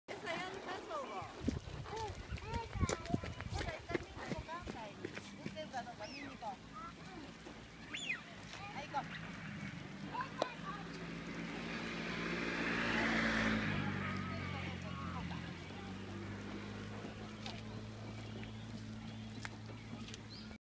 橿原神宮の鴨は、人間が餌をくれると思って追い回してくる。
奈良県の橿原神宮の池にいてる鴨達。人間は餌をくれると思ってるのか、近づくと大群で追いかけ回してきます！可愛いけど、ちょっと怖い